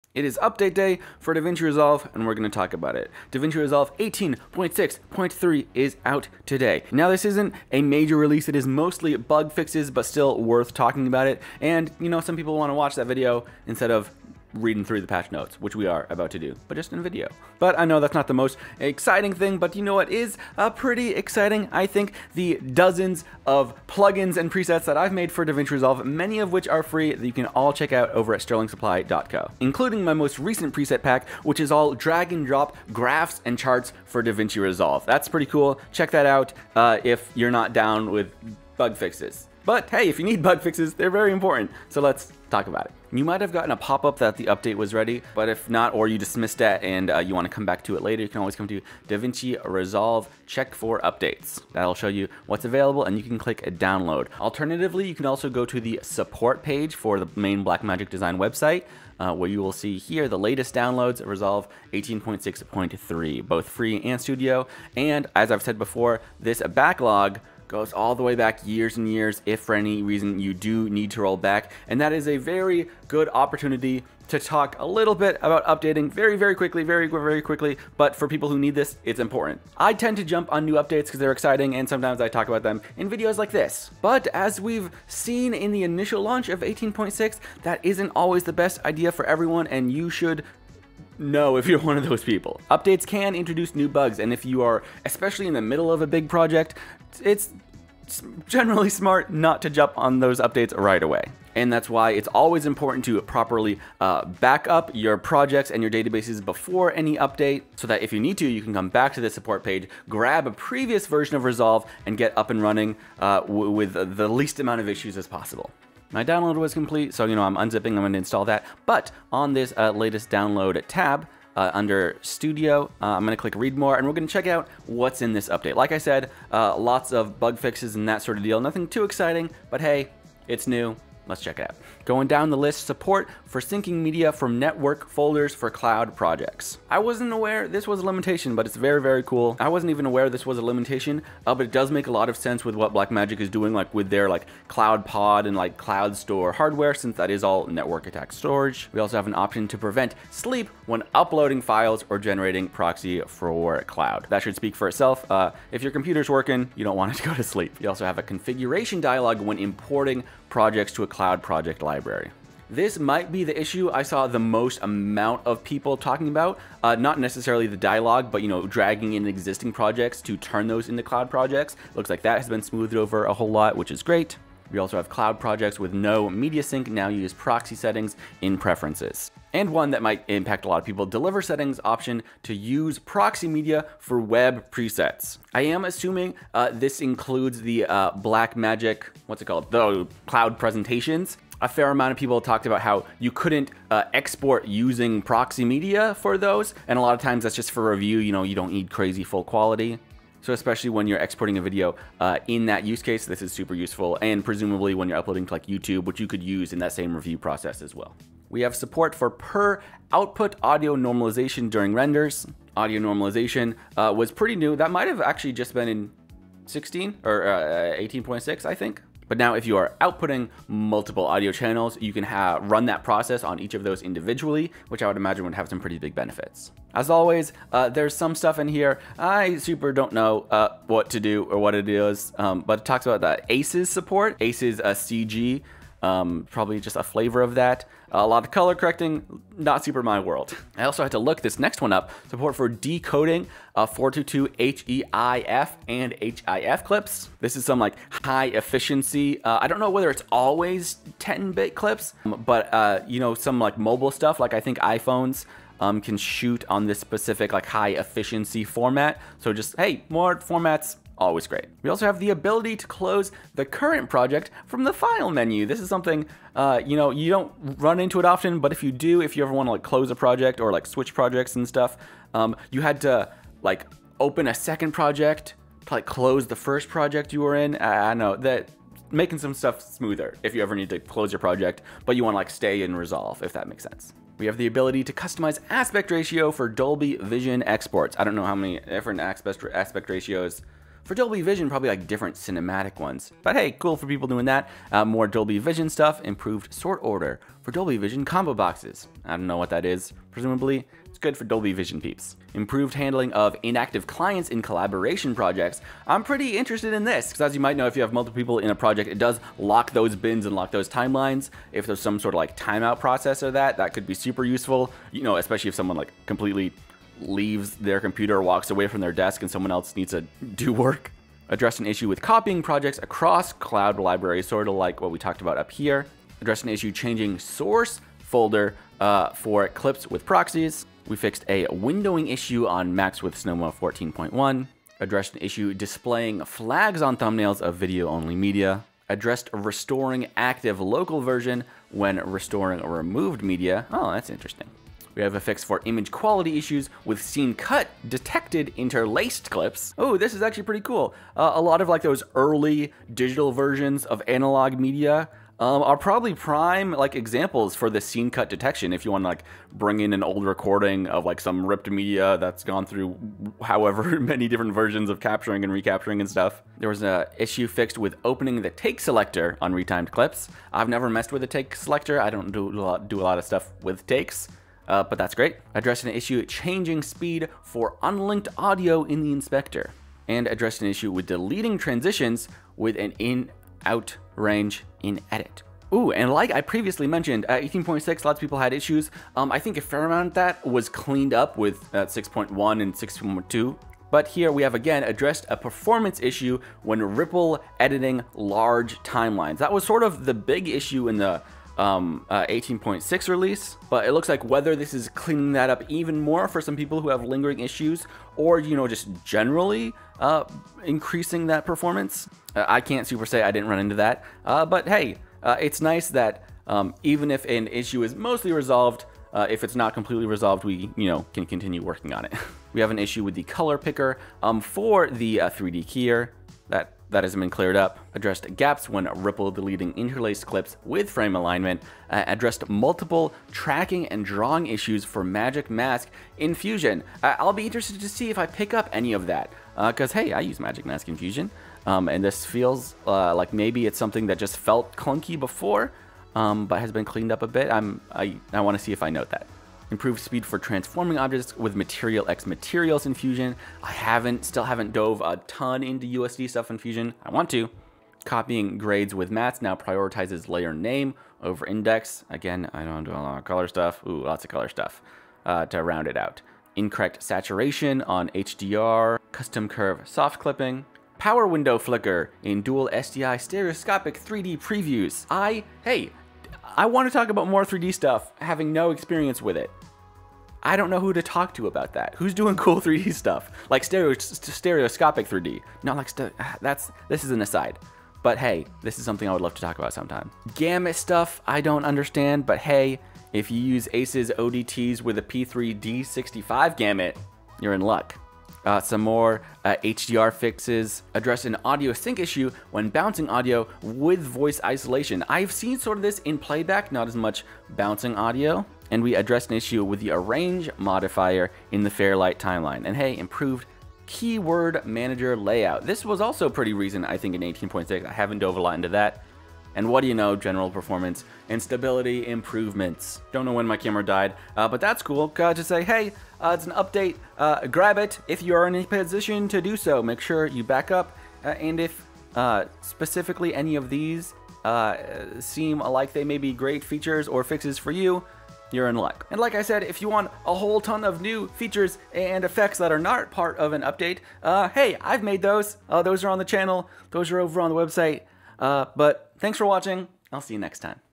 DaVinci Resolve 18.6.3 Update Notes - Bug Fixes!
DaVinci Resolve 18.6.3 is here and has some great bug fixes and minor updates! let's check out the update notes.